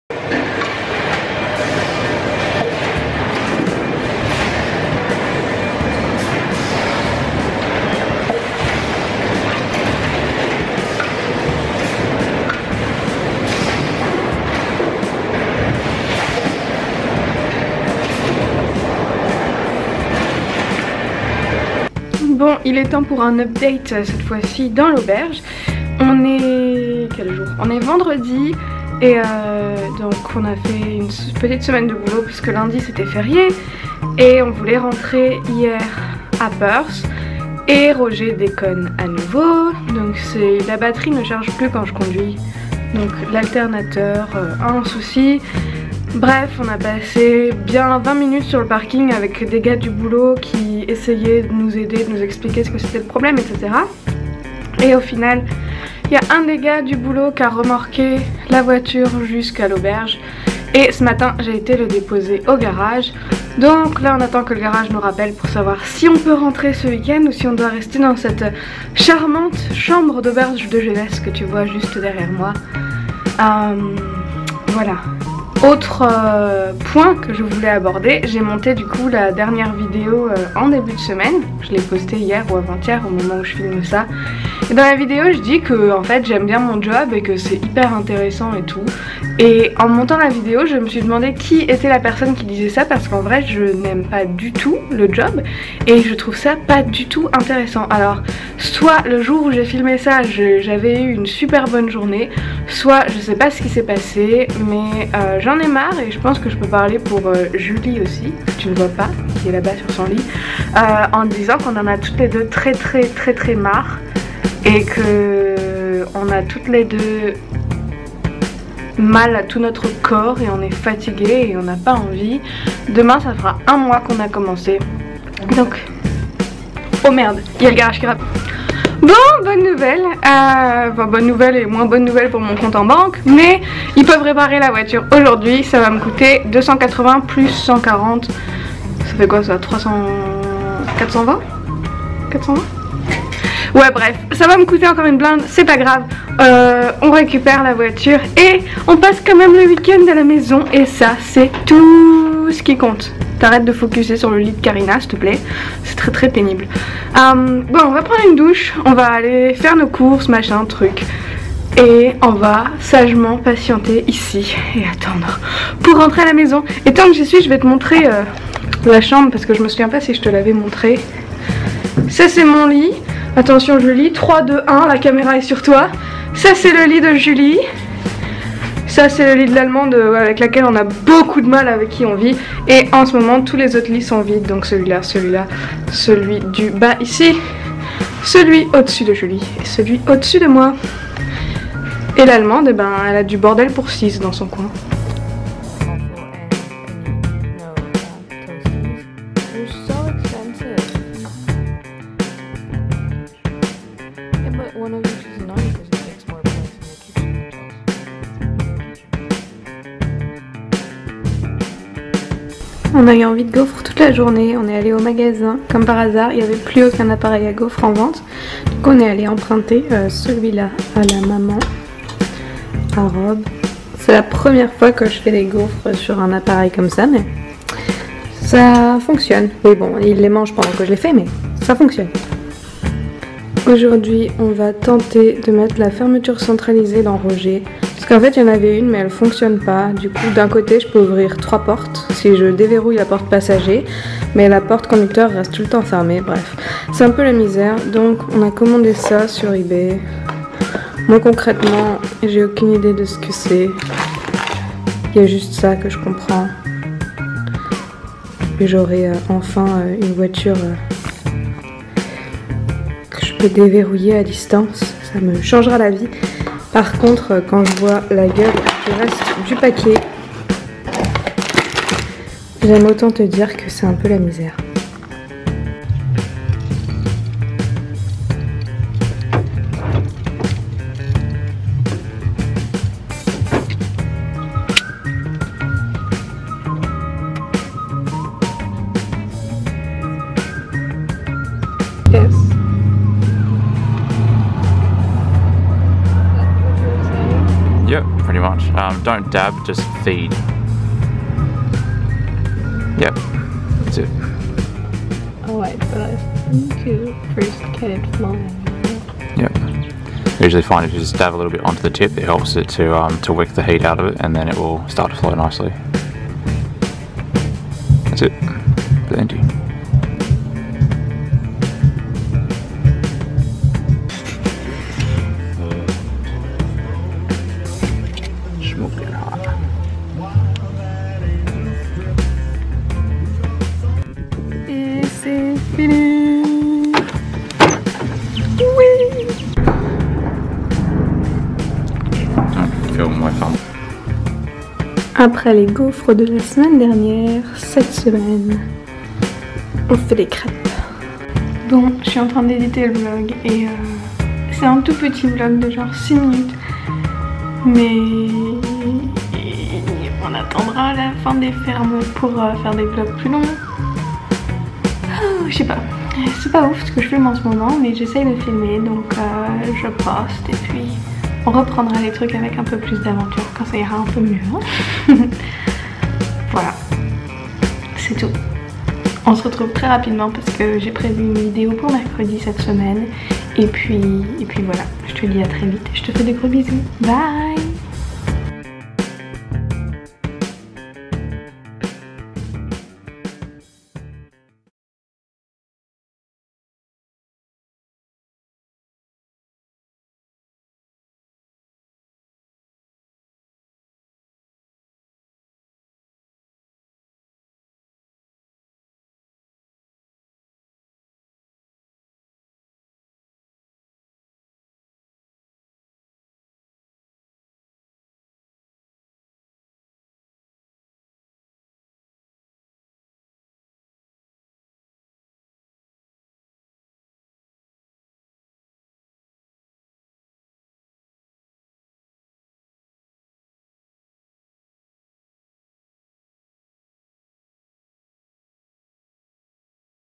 Problèmes avec Roger et update à la scierie - Weekly vlog 28
Petit vlog pour te tenir au courant ! J'espère que ça te fait plaisir !

Pour t'inscrire à la newsletter du dimanche